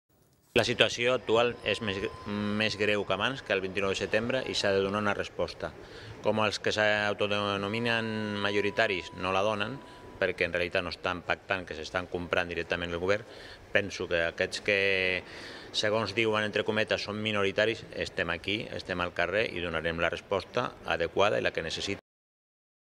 Convocatoria Vaga 27 Gener 2011
Convocatoria Vaga general 27 gener 2011 pel company Juanma de CNT Catalunya.
Vaga convocada per CNT Catalunya, Coordinadora Obrera Sindical i CGT Catalunya